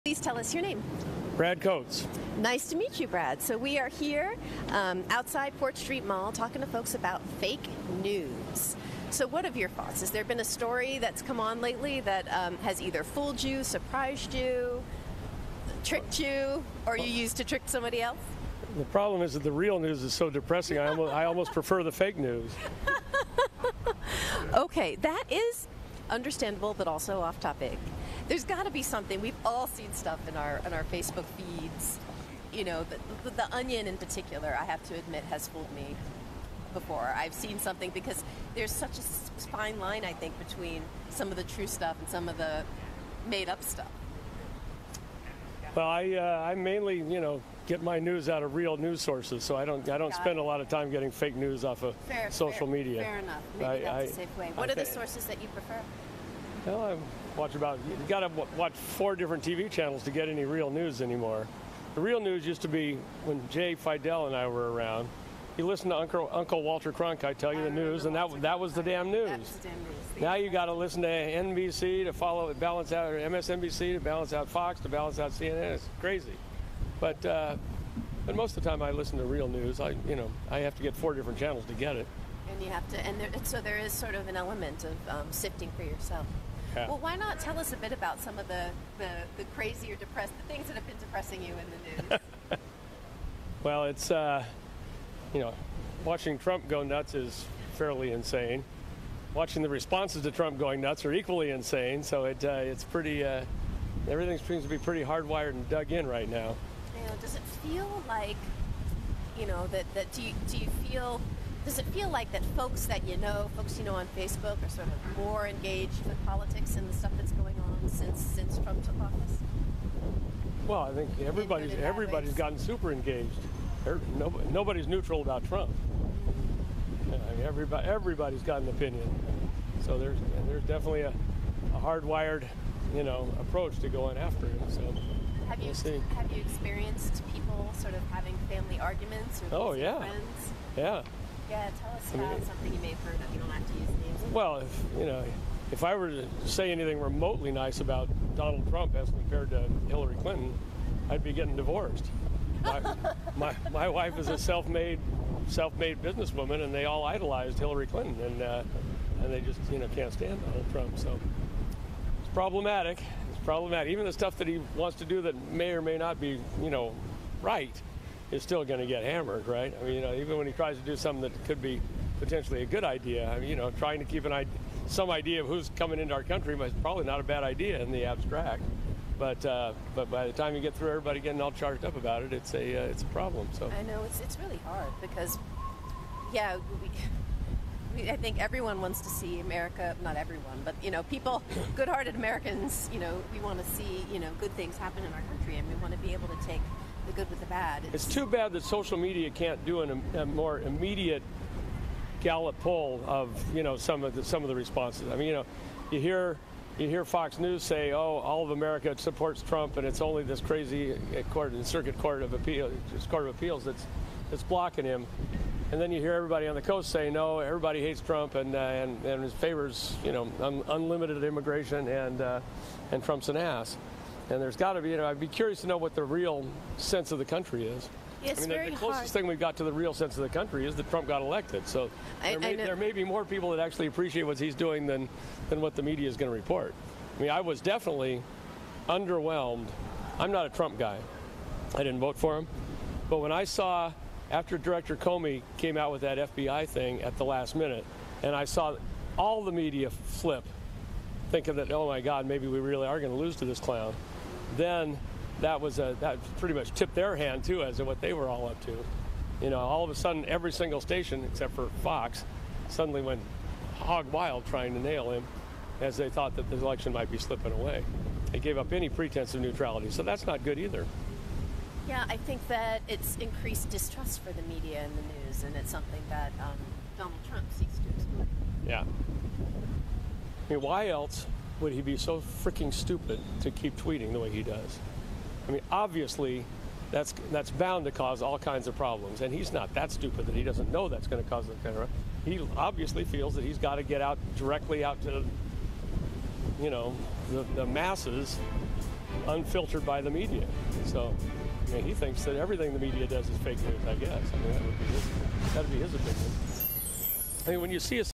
Fake News in Review
Raya Salter asks Fort Street Mall about fake news and changes in media consumption under the influence of the Trump Administration.